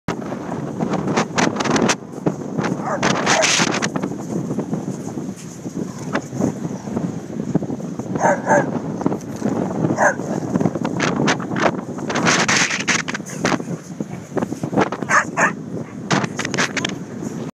Keary en kelwyn
Samen stoeien met zijn broertje kelwyn